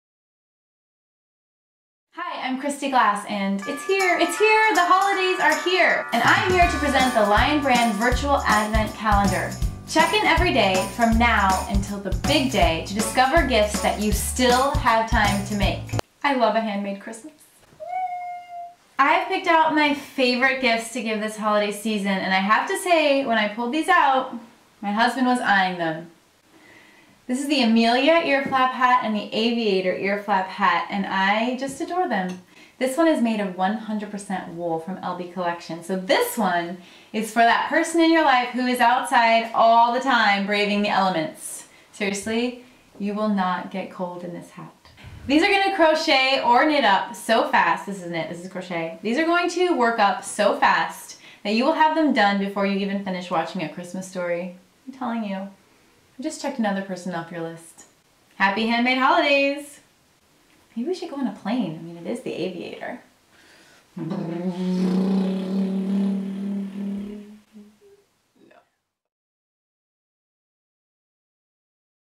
Knit an Amelia Earflap Hat! Lion Brand's Handmade Holiday Countdown #23
HANDMADE HOLIDAY COUNTDOWN - DAY TWENTY-THREE!

Over the next 30 days, Kristy Glass from the Glass Posse channel will share a free knit or crochet pattern each day, leading up to the holiday season!

1) Get the free knit Amelia Earflap Hat pattern from this video